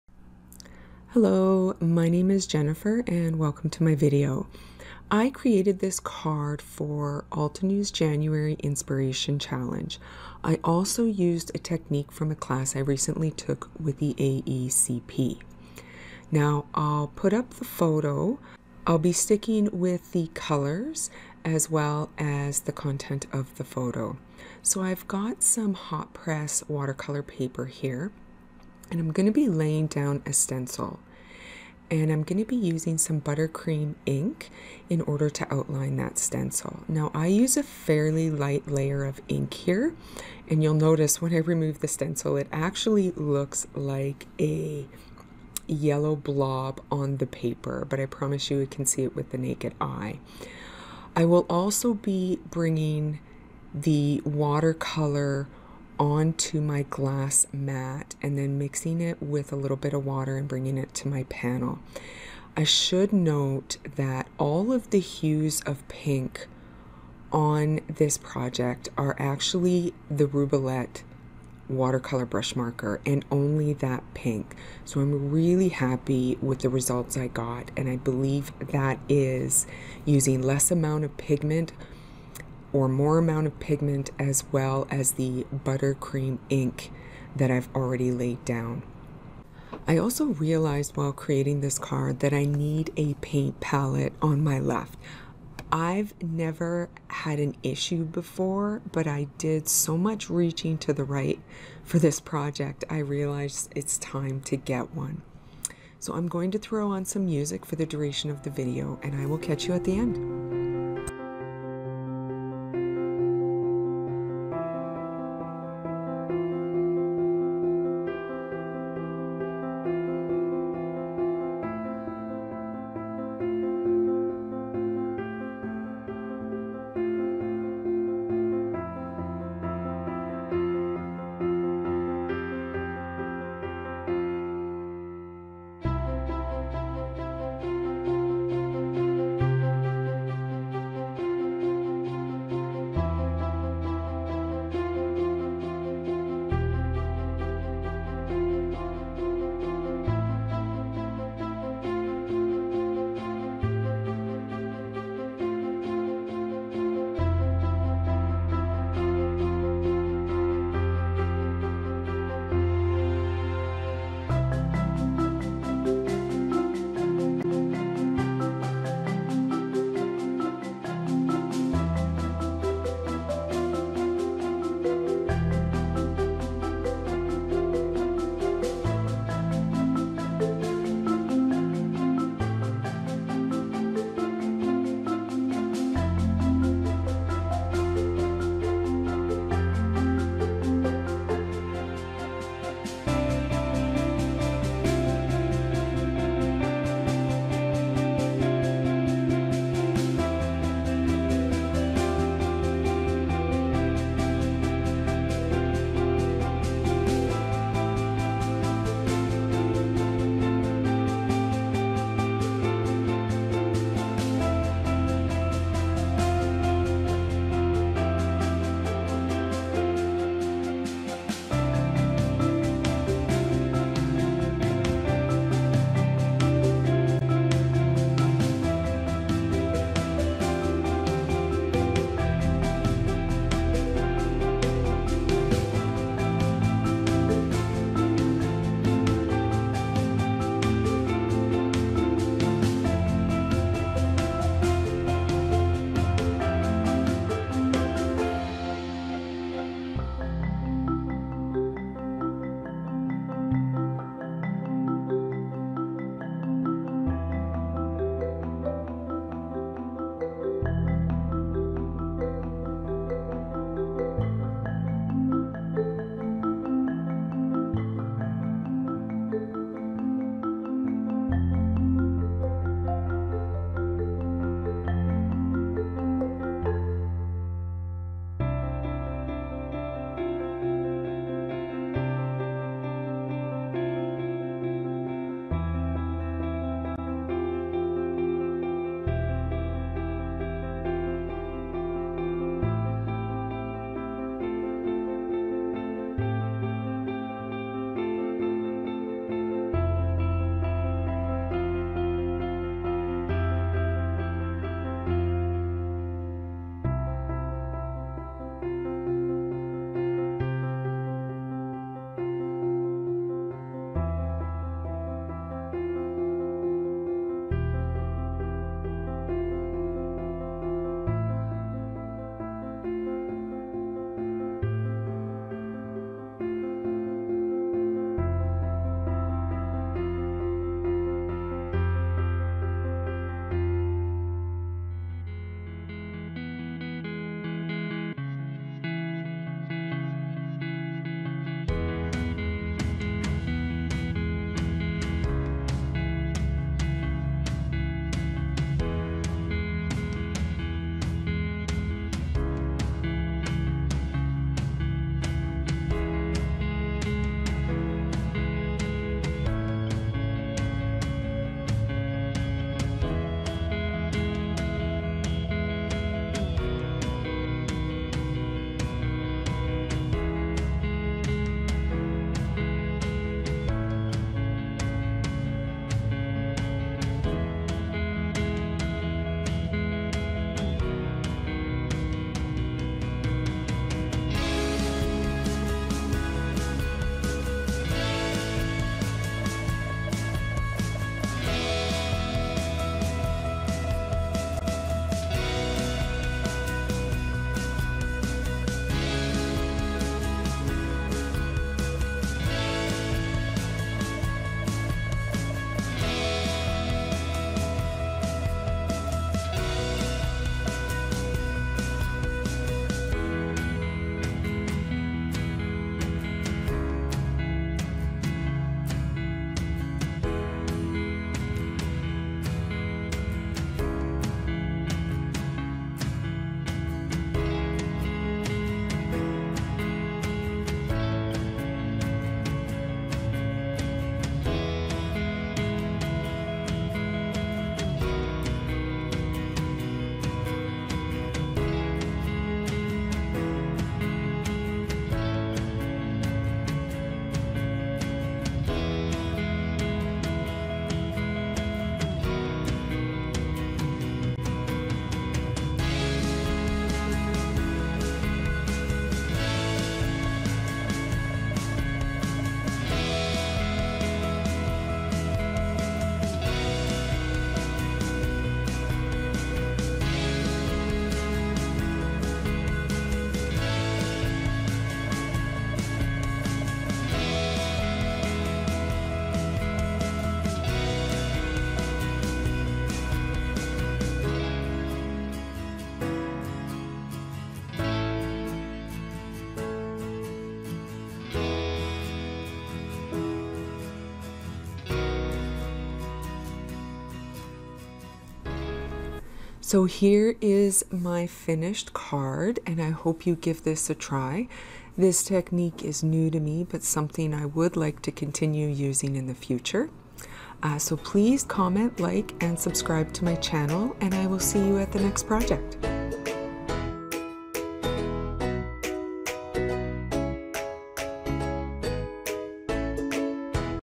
Challenge Yourself - Watercoloring and Stencil
For this video I used Altenew's January inspiration challenge to create this card. I also used a technique from one of my AECP classes - watercoloring stencils. I like these types of challenges, as it keeps my mind from using the same ideas and colors for my cards.